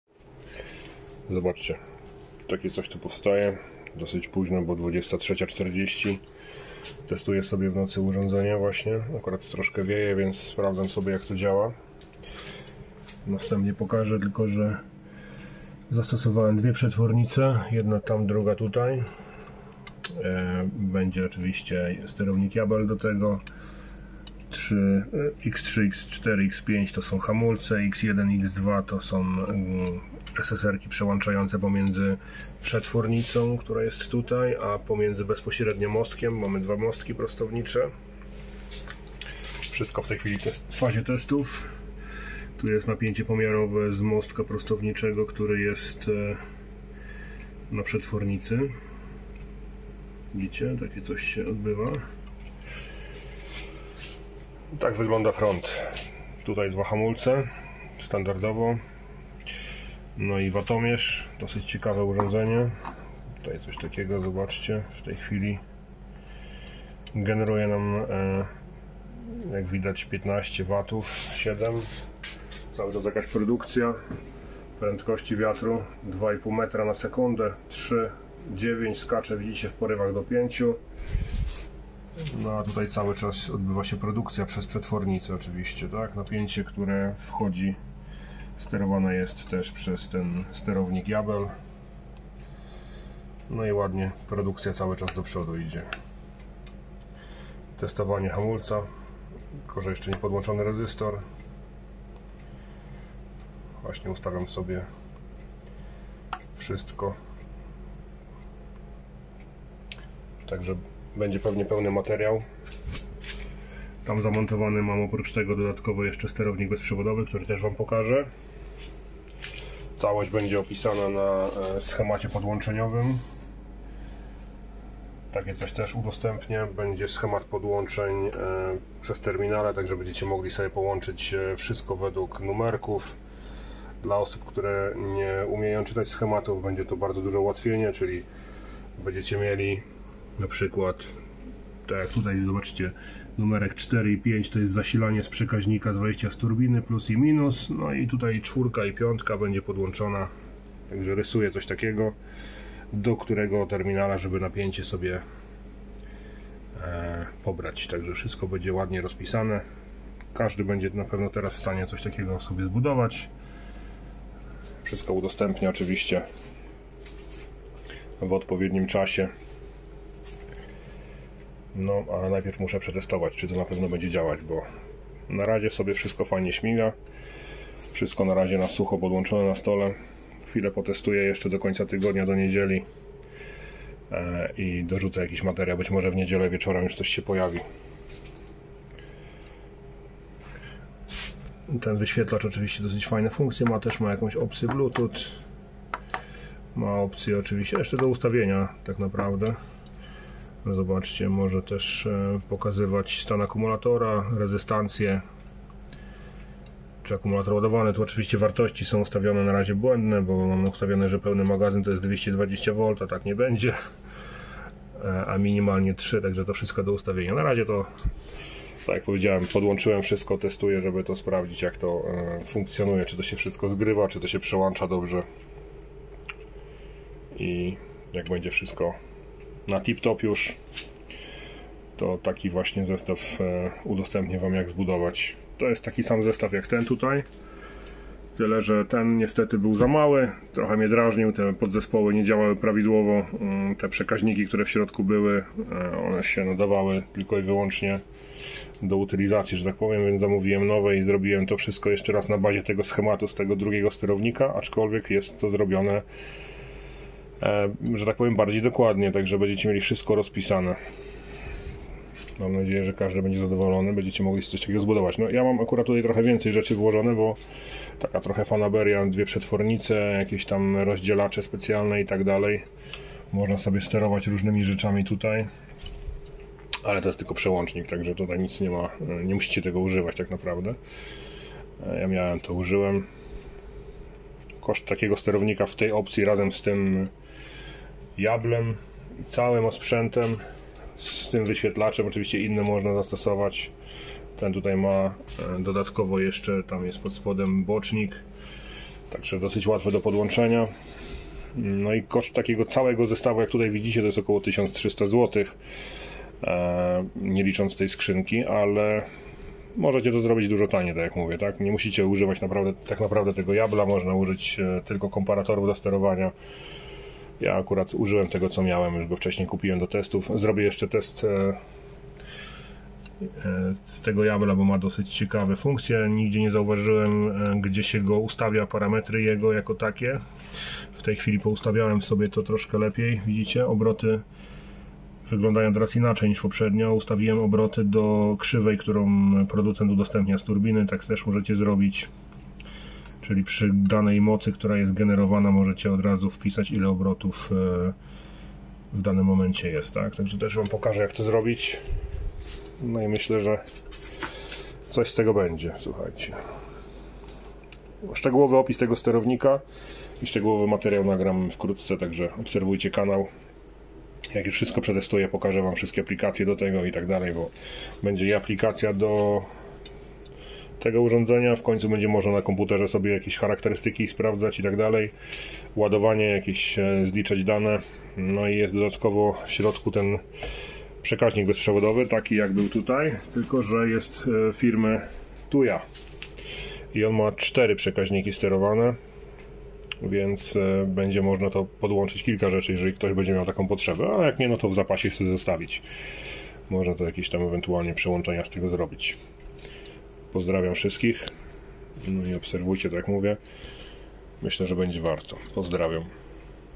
Turbina wiatrowa w akcji - Test nowego sterownika przy słabym wietrze | Ładowanie magazynu energii.
W tym filmie przedstawiam nocne testy turbiny wiatrowej oraz nowego sterownika, który znacznie poprawia wydajność ładowania magazynu energii nawet przy słabym wietrze. Zastosowałem podzespoły wyższej jakości, co zaowocowało lepszą wydajnością w porównaniu do poprzedniego rozwiązania. W filmie pokazuję również kilka modyfikacji w połączeniach, które wprowadziłem, aby zoptymalizować pracę systemu. Jeśli interesuje Cię energia odnawialna, magazyny energii oraz jak skutecznie ładować baterie przy niskiej prędkości wiatru, ten materiał jest dla Ciebie!

Chcesz wesprzeć rozwój kanału? Kup mi wirtualną kawę!

Dołącz do naszej społeczności na Facebooku i podziel się swoimi doświadczeniami: ➡️